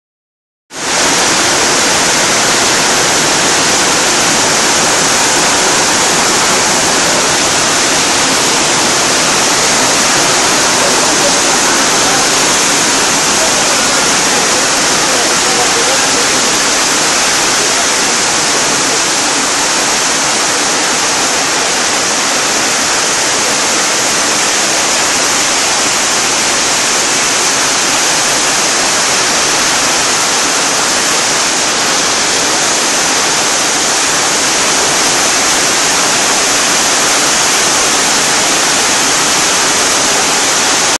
Rain in the rainforest - near Tena, Ecuador
Went for an afternoon jungle hike but stopped for half an hour in these half constructed jungle cabins until the storm passed.